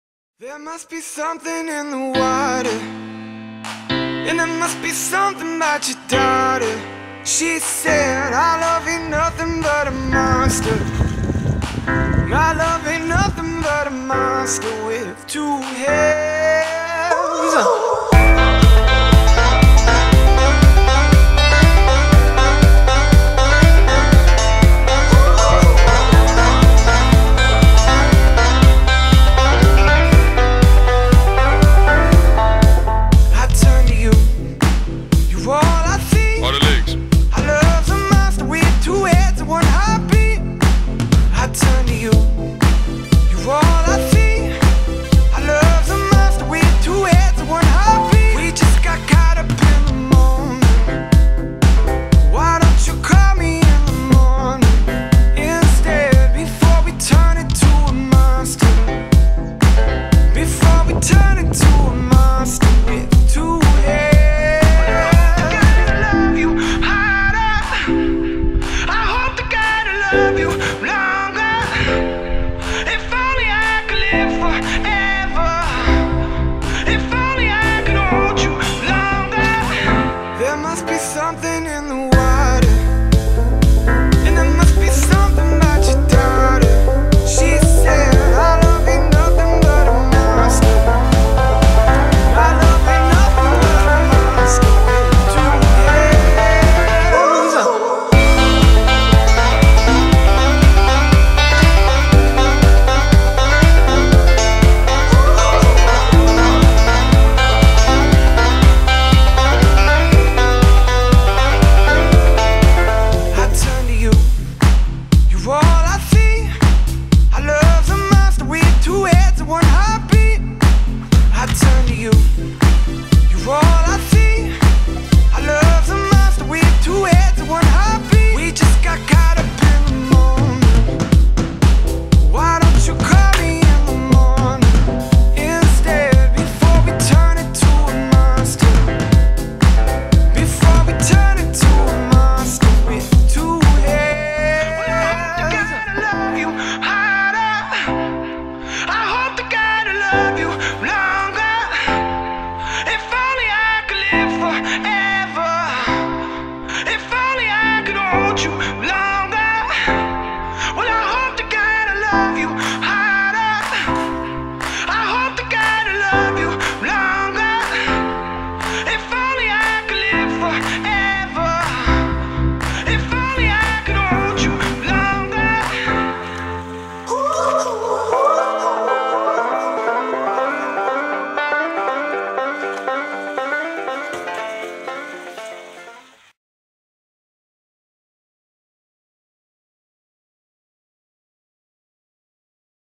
Warriors Polo Tips 9 - Baulking and Shooting
A few dos and don'ts for water polo shooting and baulking techniques.

Thanks to Warriors players Matt Jacobson, Josh Faber, Ollie Schuurmans and Keegan Clark for braving the cold and sticking to their training!

Filming and editing - Dean Rimmer